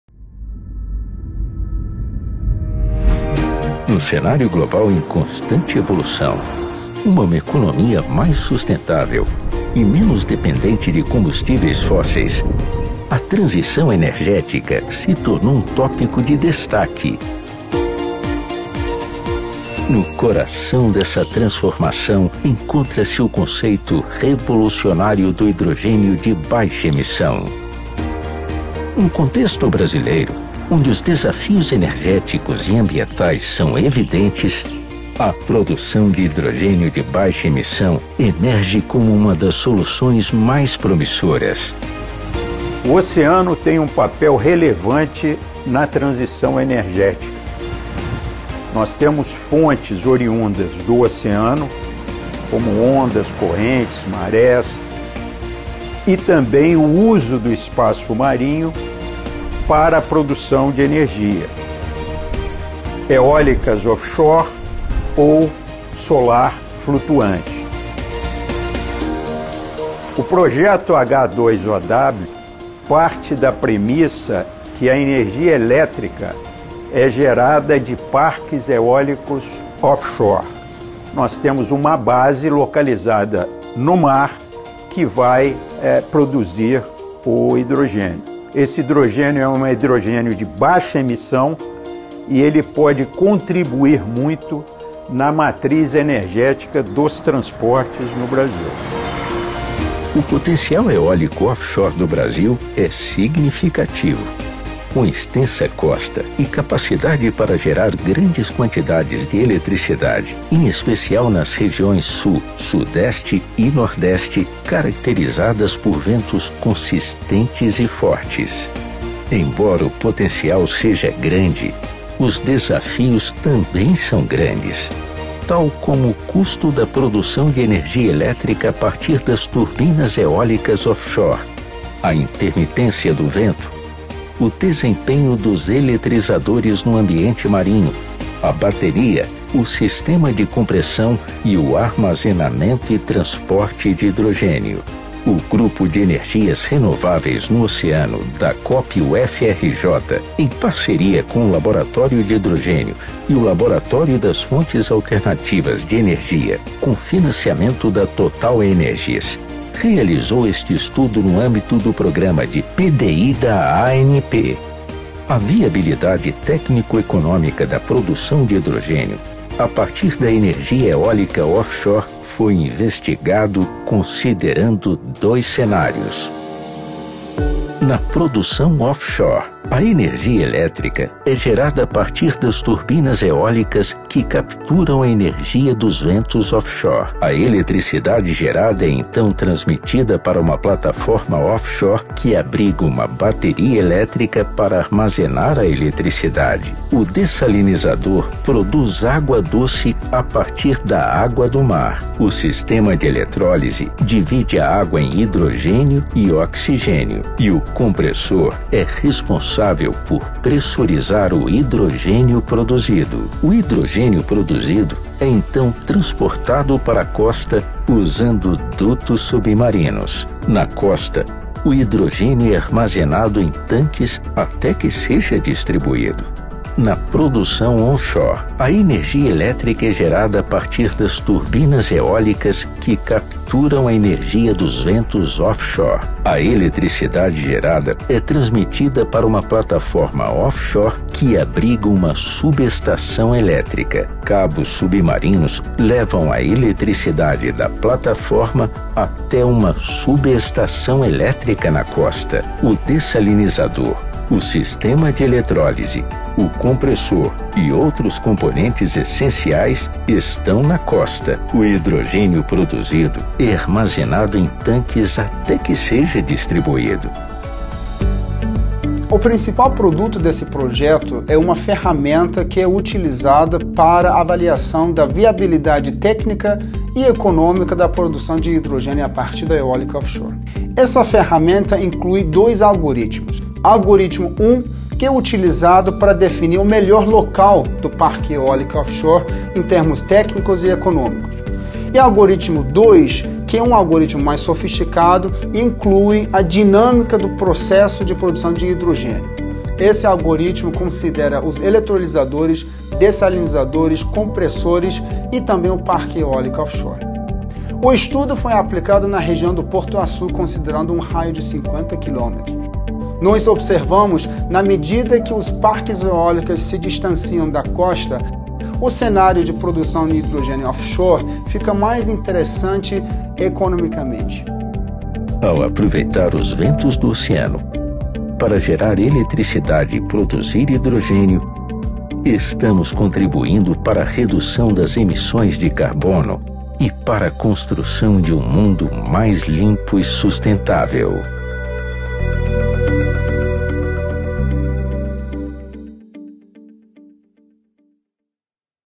Produção de hidrogênio a partir da eólica offshore - Brasil
O Grupo de energias renováveis no oceano (GERO) da Coppe UFRJ em parceria com o laboratório de hidrogênio (Labh2) e o laboratório das fontes alternativas de energia (LAFAE) com o financiamento da TotalEnergies, realizou este estudo no âmbito do Programa de P&D da ANP.

Neste vídeo você consegue ver a fundo sobre a investigação que considerou 2 cenários da viabilidade técnico econômica da produção de hidrogênio a partir da energia eólica offshore.

Assista até o final e entenda melhor sobre o assunto!